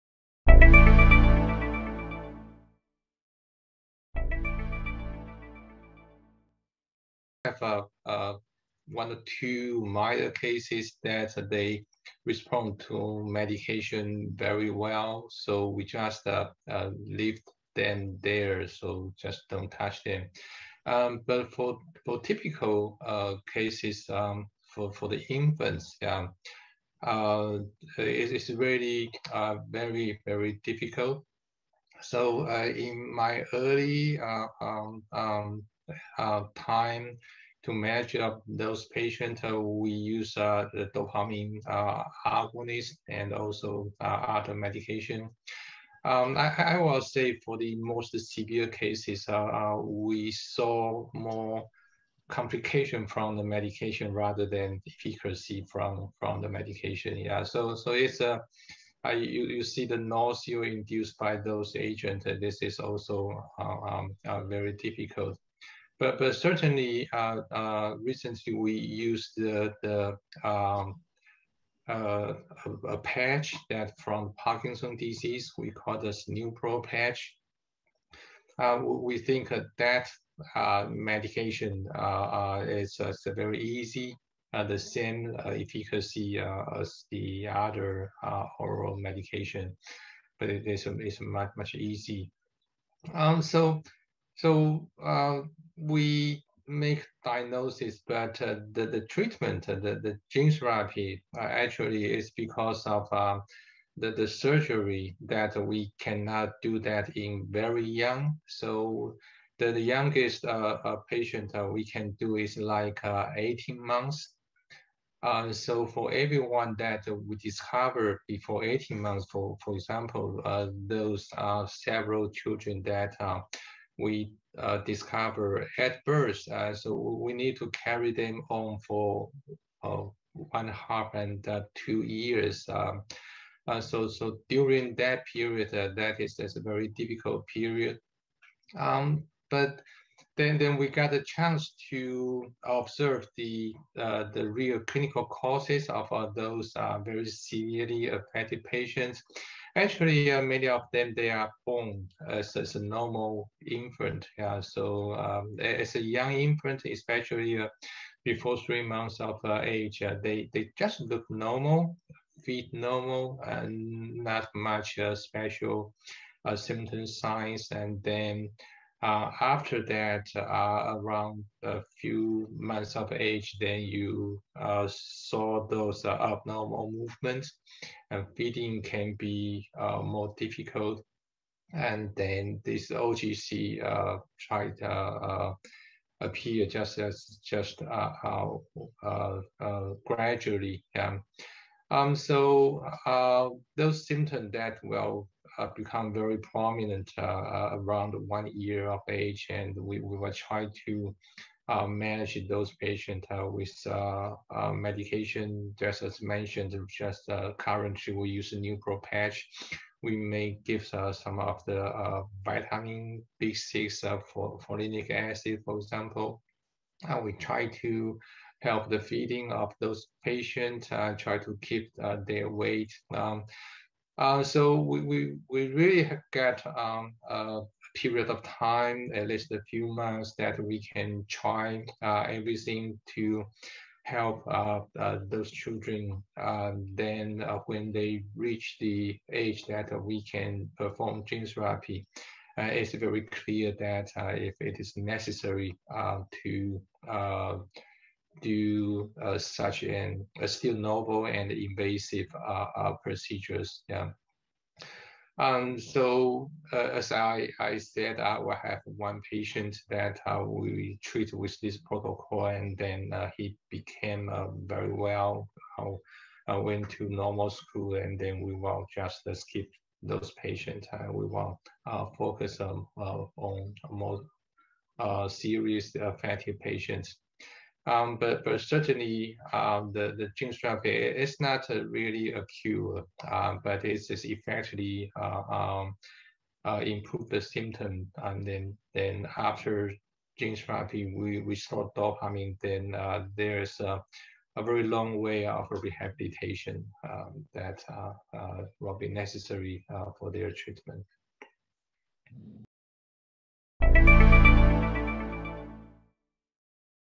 AADC Deficiency Treatment
As part of this expert roundtable, Paul Wuh-Liang Hwu, MD, PhD of the Taipei National University Hospital, described current approaches to treatment of aromatic L-amino acid decarboxylase (AADC) deficiency.

Dr. Hwu said that for typical infant cases, it is very difficult to treat their symptoms. It is possible to use dopamine-based medications to help manage some patients, “but for the more severe cases,” Dr. Hwu noted, “we saw more complications from the medication” and less benefit. More recently, he has utilized a rotigotine transdermal patch (used to treatment Parkinson’s disease), which he finds easier to use than oral dopamine agonists.

Patients don’t often show signs of AADC deficiency until they are older than 3 months. Once those signs of abnormal movement do appear, feeding can become more difficult. He generally tries the rotigotine patch, along with vitamin B6, and focuses on improving the feeding of those babies and on their weight. Once they reach about 18 months of age, they can consider the newest modality—gene therapy (Note: gene therapy is still under clinical investigation and is not yet approved by the US Food and Drug Administration for the treatment of AADC deficiency).

Dr. Hwu described gene therapy as “not really a cure, but actually a way of improving symptoms.” Once dopamine levels are restored, then patients have an extended period of rehabilitation, where they slowly recover their ability to control abnormal movement and improve their dystonia.

Philip L. Pearl, MD
Director, Epilepsy and Clinical Neurophysiology, Boston Children’s Hospital
William G. Lennox Chair and Professor of Neurology, Harvard Medical School
Boston, MA

Warren A. Marks, MD
Medical Director, Movement Disorders
Cook Children’s Jane and John Justin Neurosciences Center

Paul Wuh-Liang Hwu, MD, PhD
Professor, Department of Pediatrics and Medical Genetics
National Taiwan University Hospital
Tapei, Taiwan

Irina A. Anselm, MD
Director of the Mitochondrial Program and Co-Director of the Neurometabolic Program, Boston Children’s Hospital
Assistant Professor of Neurology, Harvard Medical School
Boston, MA

Jennifer O’Malley, MD, PhD
Clinical Assistant Professor, Neurology & Neurological Sciences, Stanford Medicine
Pediatric Neurologist, Stanford Children’s Health
Stanford, CA